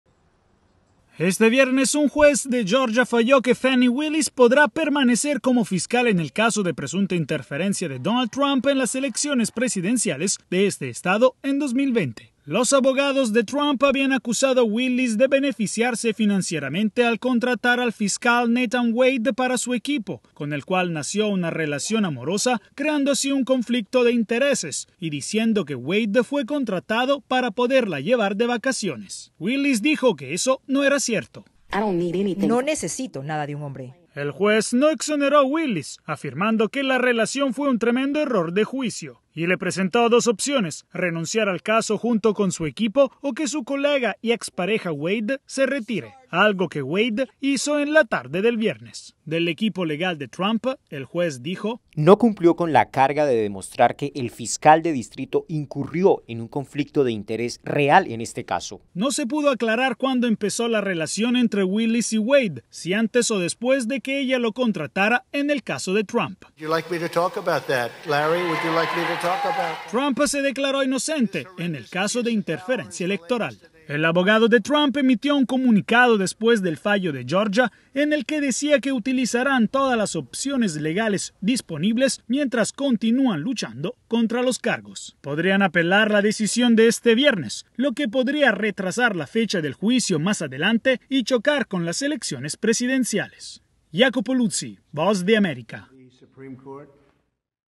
Fiscal podrá seguir a cargo del caso contra Trump en Georgia
La fiscal Fani Willis podrá continuar a cargo del caso por presunta interferencia electoral contra el expresidente Donald Trump en Georgia. Este viernes, un juez falló que no hubo ningún conflicto de intereses en su relación con un subordinado, el cual tuvo que renunciar. Iacopo Luzi, periodista de la Voz de América, informa.